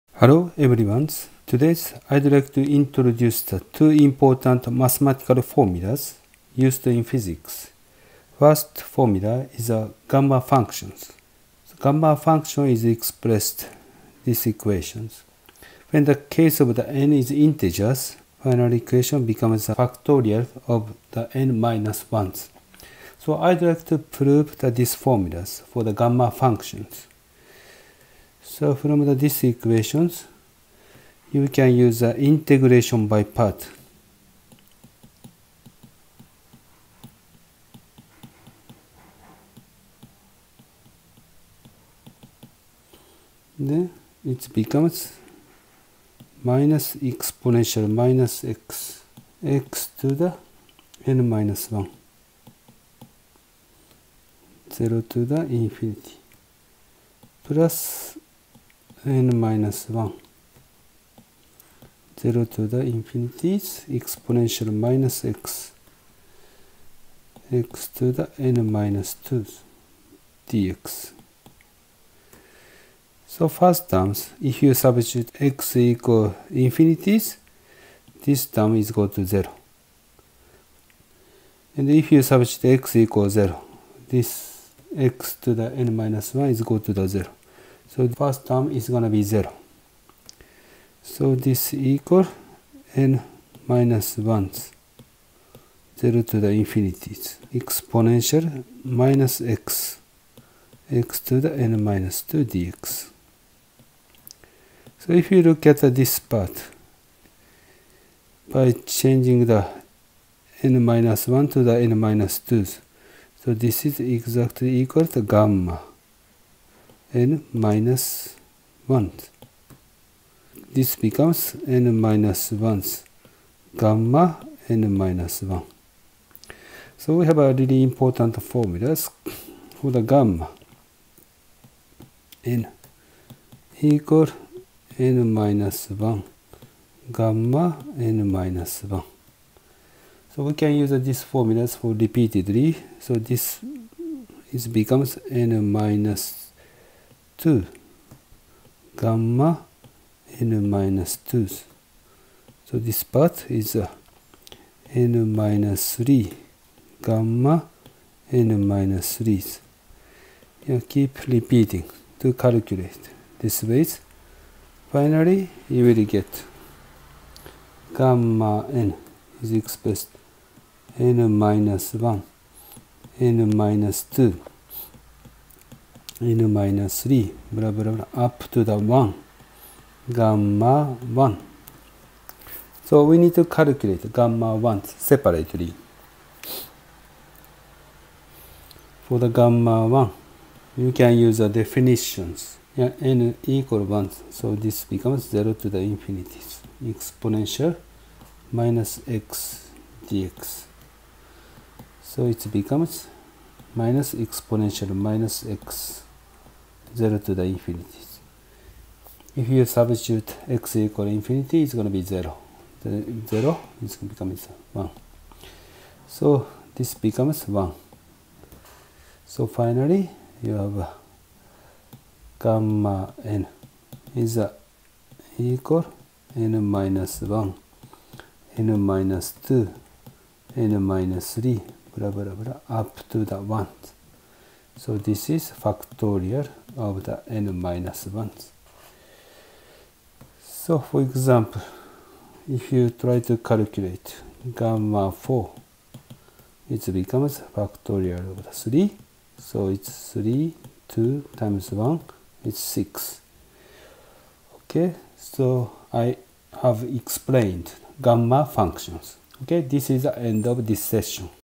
II.2.1. Elementary Physics in Stars (Gamma Function)
We introduce some important functions. At first, the Gamma function is introduced in this lecture.